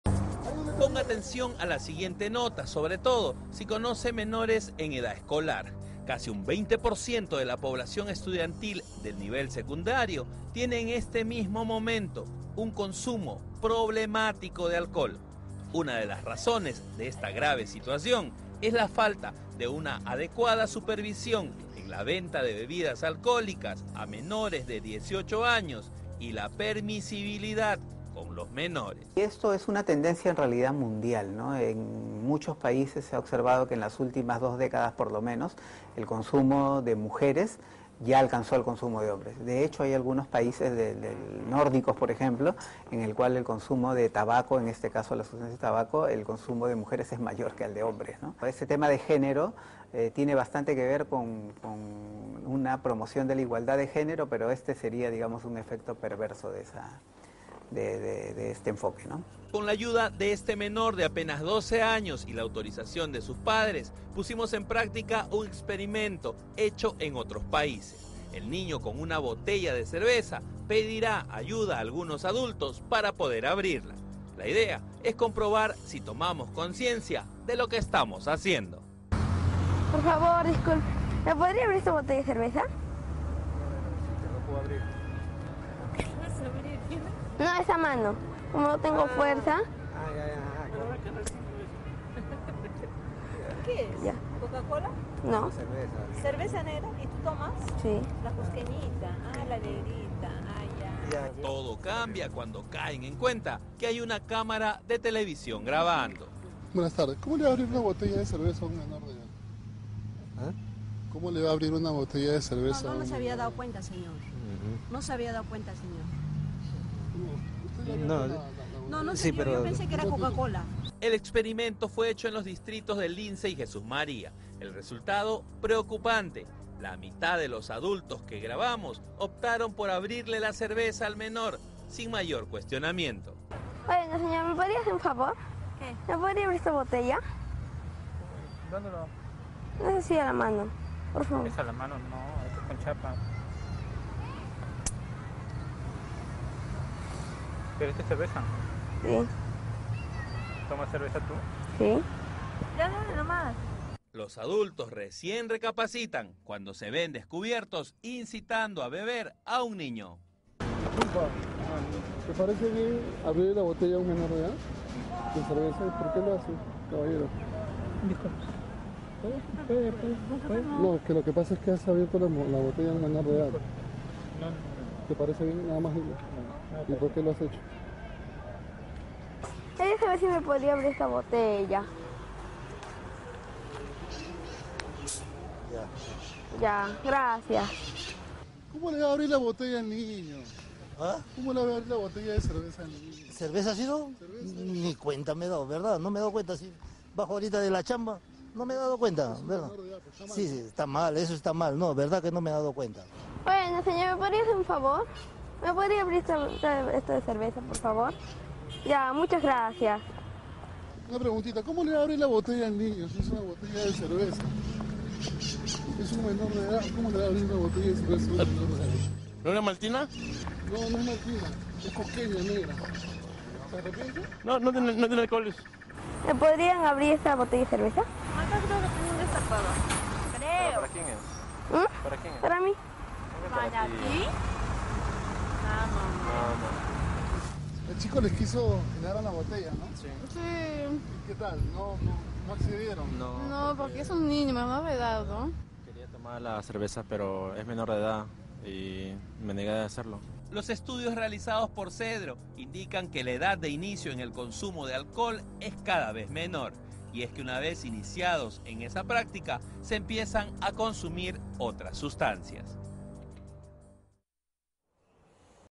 ¿Le abrirías una botella de cerveza a un niño?
Según estudios, la edad de inicio en el consumo de bebidas alcohólicas es cada vez menor y cuando el adolescente se ve envuelto en esta práctica, empieza a consumir sustancias dañinas para su salud.